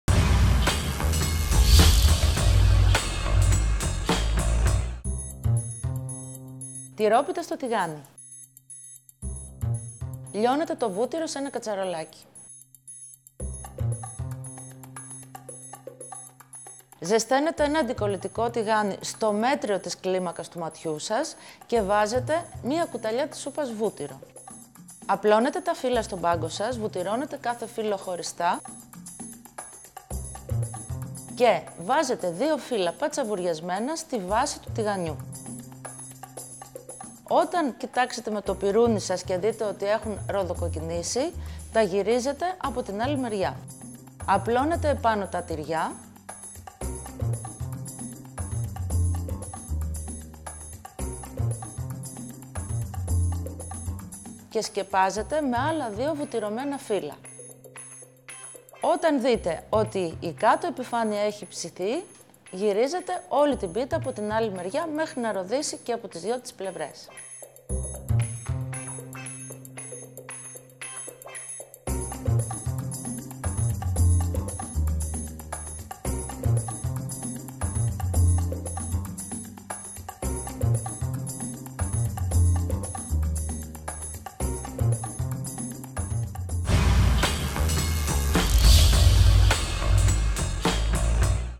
Τυρόπιτα στο τηγάνι | Ελένη Ψυχούλη | Olivemagazine.gr
🍽 Olive Magazine recipes 🍽

Μια τυρόπιτα της ευκολίας, που γίνεται μέσα σε ελάχιστα λεπτά, αρκεί να έχετε στο ψυγείο σας φύλλα κρούστας και νόστιμα τυριά.